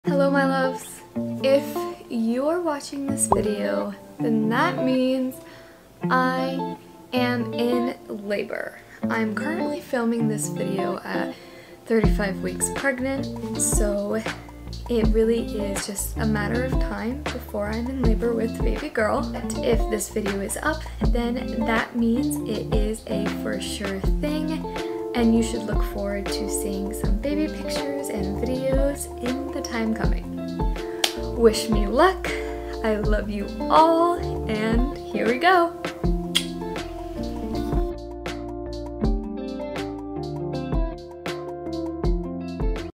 If You're Watching This I'm In Labor
If You're watching this that means Im in labor! Baby girl will be here soon... I cant wait to share this journey with you!